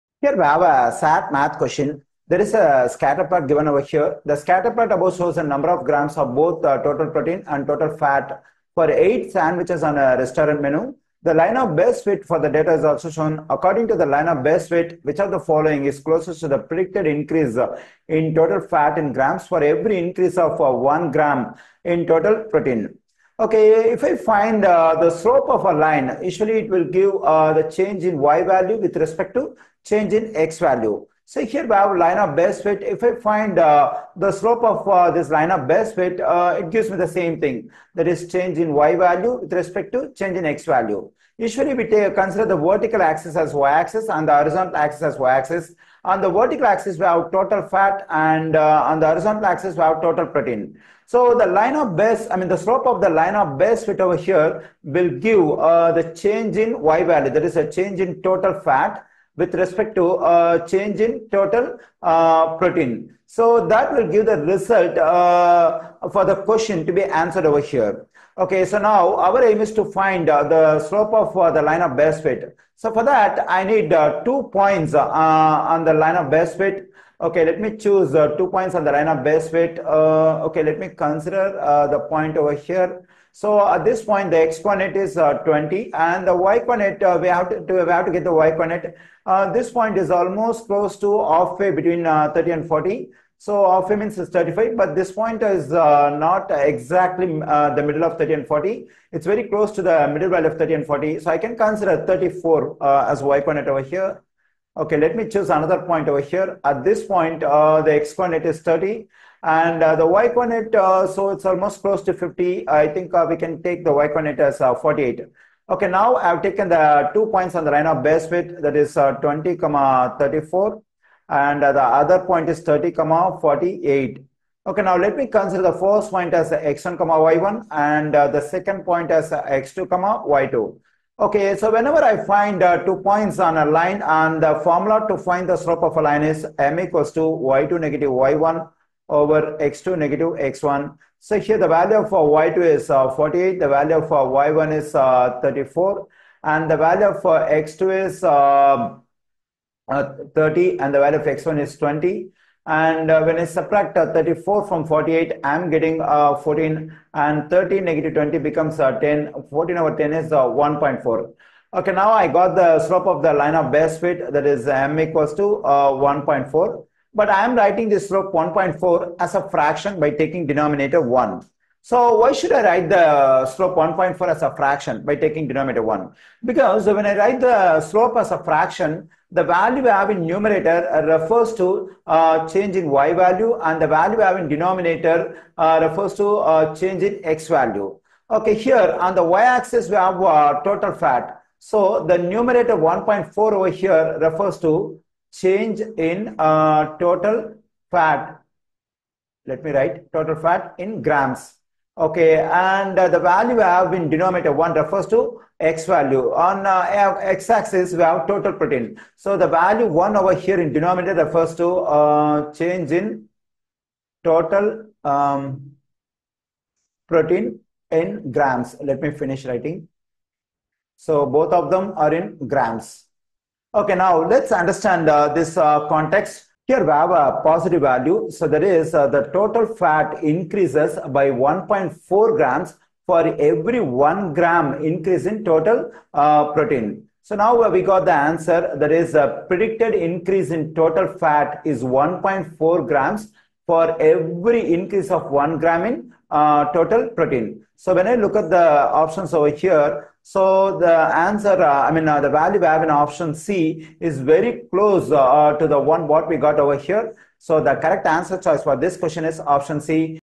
SAT Math (Data Analysis)
The scatterplot above shows the numbers of grams of both total protein and total fat for eight sandwiches on a restaurant menu. The line of best fit for the data is also shown. According to the line of best fit, which of the following is closest to the predicted increase in total fat, in grams, for every increase of 1 gram in total protein?